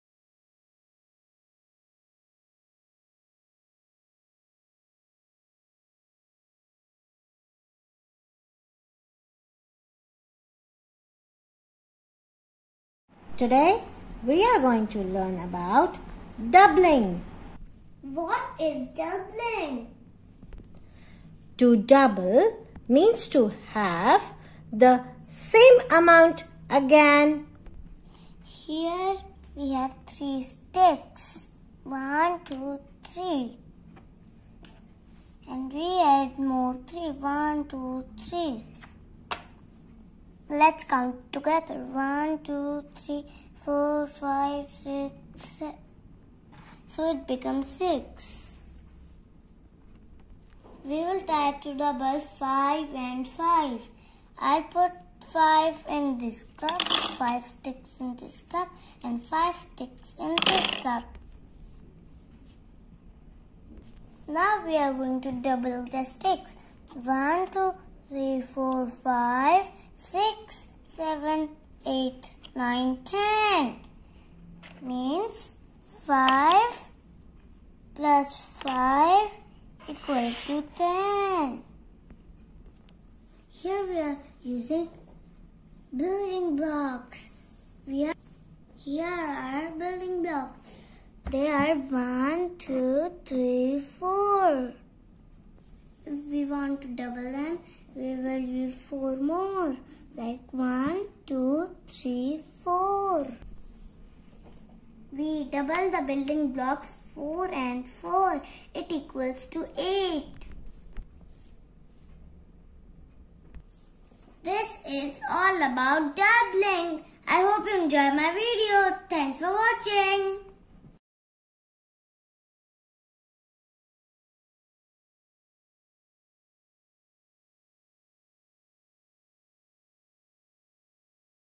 Doubling for kids | Teaching doubles | Doubling numbers song | Double and half
Doubling numbers means to have the same amount again.

2+2=4
3+3=6
4+4=8

With this video, we have tried to teach and learn the concept of doubling numbers or double numbers. we have used some objects to learn.

Our social Handles:
Please Follow me on these social media platforms:
Thanks

Thanks ❤